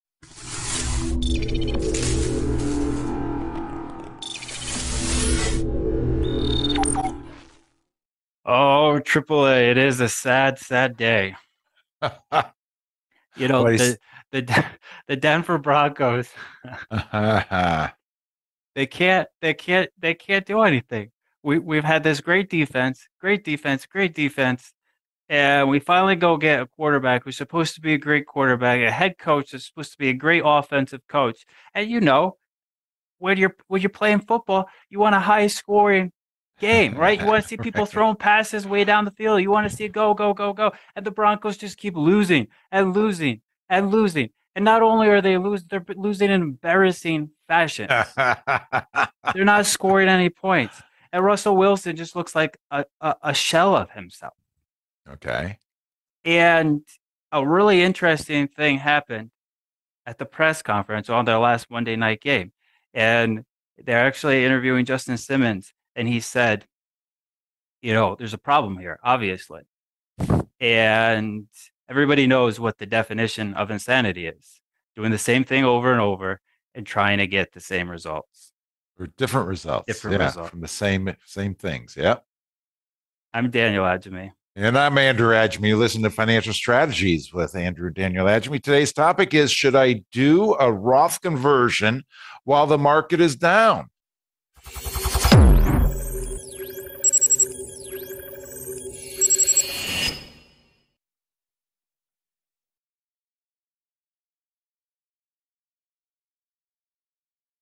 Should I Do a Roth Conversion While the Market is Down? Sizzle Reel
Recent stock market drops have made Roth Conversions more appealing for many. However, you need to weigh the upfront taxes, along with further consequences you may not (yet) be aware of.